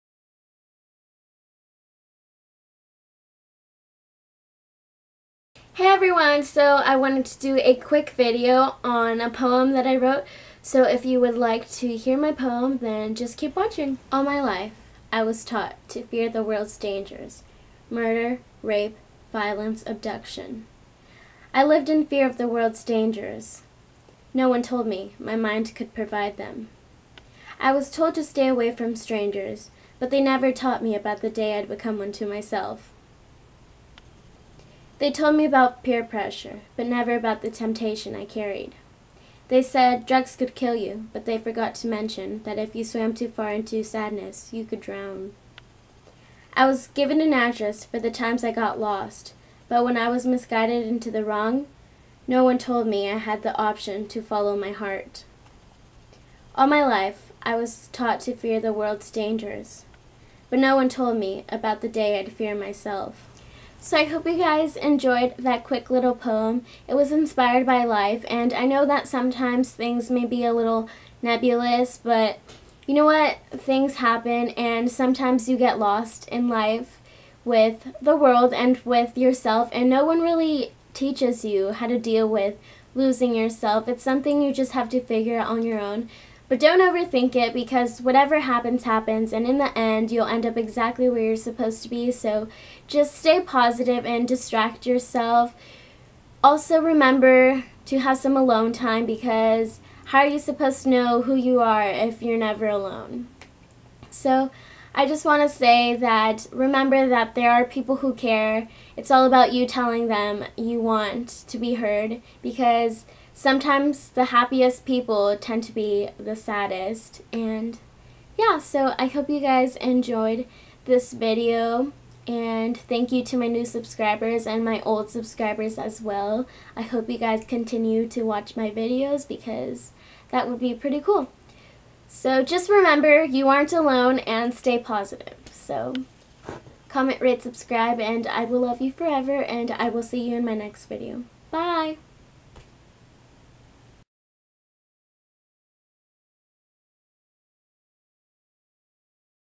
dangers of the world
I came up with this poem in math class and I really like how it turned out and I hope you did too. I am by no means trying to offend anyone or belittle any of the words problems.